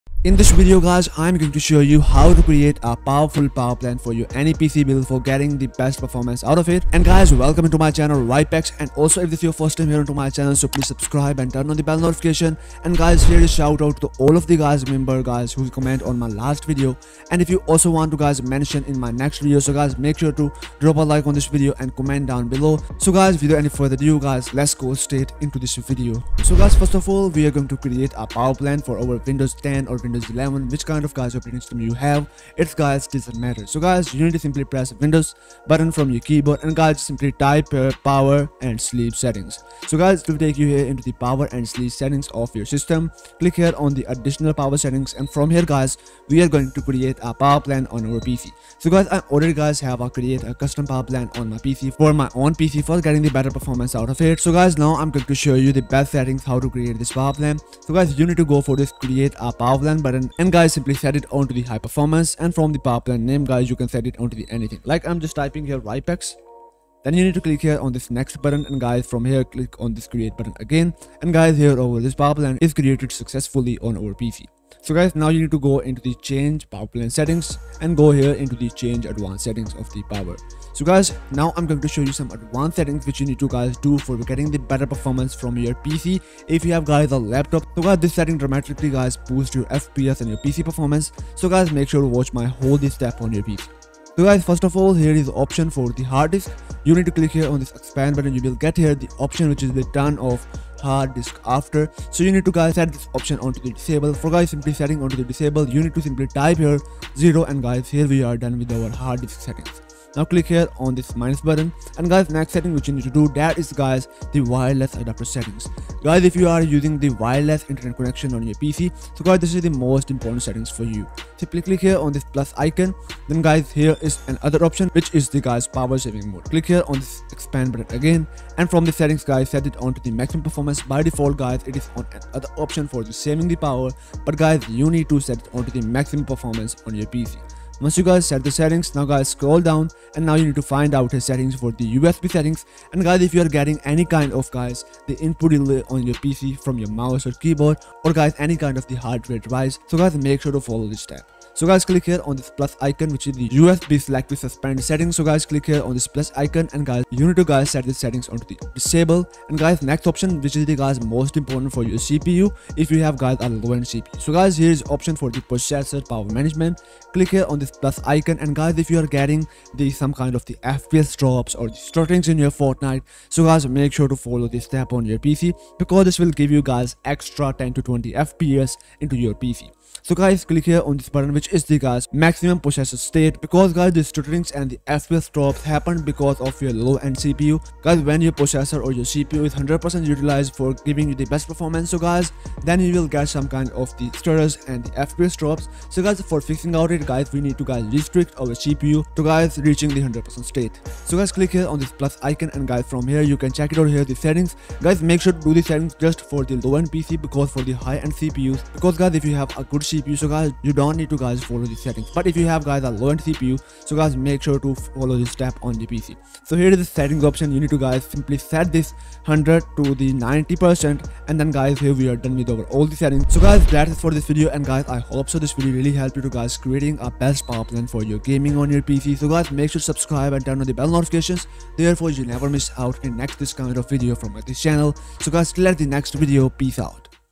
Custom Power Plan For Gaming (Best Power Plan Settings For Gaming)
Hello People!
Today in this video, I am showing you Custom Power Plan For Gaming (Best Power Plan Settings For Gaming) Which Gives You the Best Performance For Any PC Build For Gaming. If You Wants To Boost Your FPS & Increase PC Performance So Make Sure To Watch Complete Video!

Title: Custom Power Plan For Gaming (Best Power Plan Settings For Gaming)

INTRO 0:00

Shoutout 00:12
Start Optimization 00:26

OUTRO 03:42
End 3:58